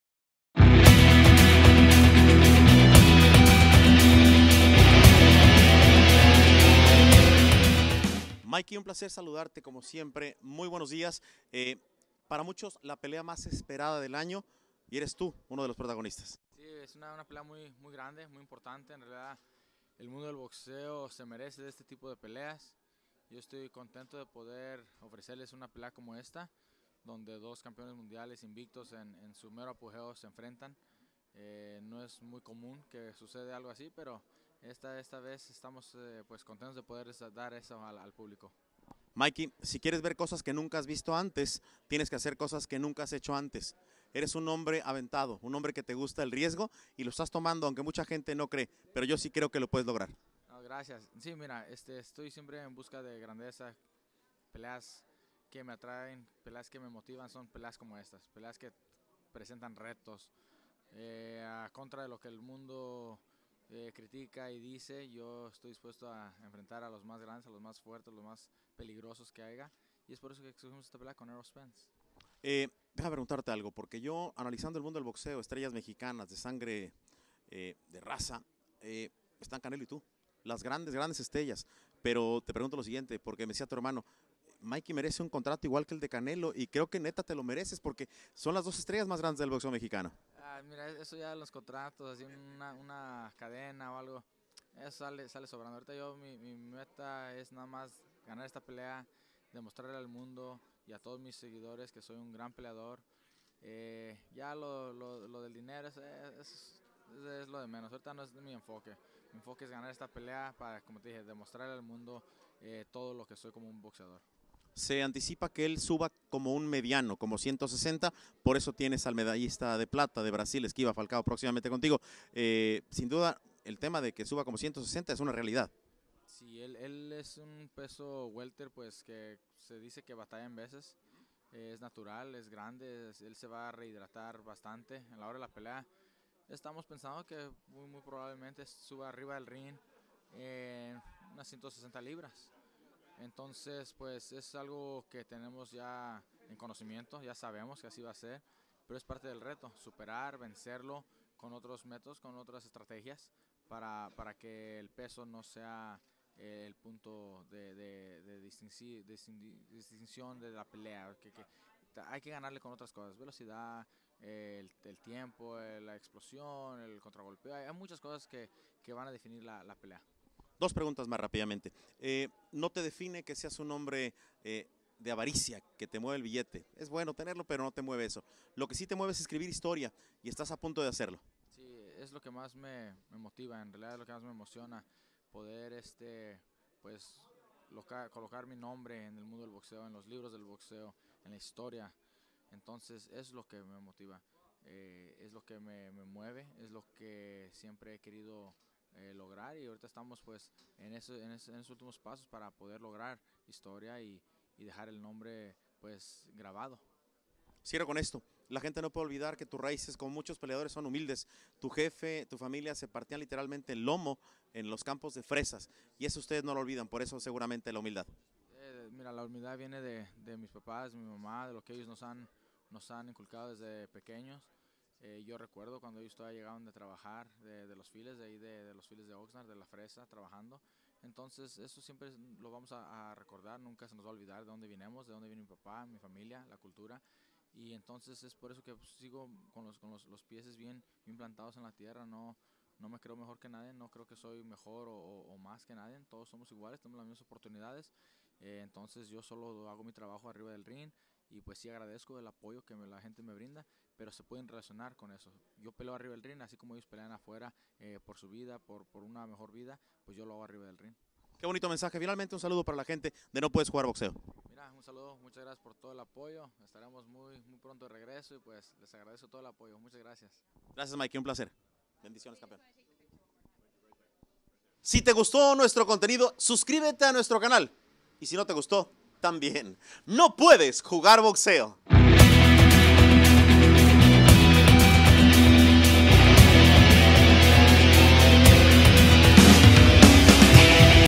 Mikey García: "Yo peleo en el ring, como mis paisanos por una mejor vida".
Mikey García analiza su pelea ante Errol Spence Jr y dice que no le importa él no ser favorito y muestra su humildad recordando sus raíces.
Productor: Hernán Pambukdjian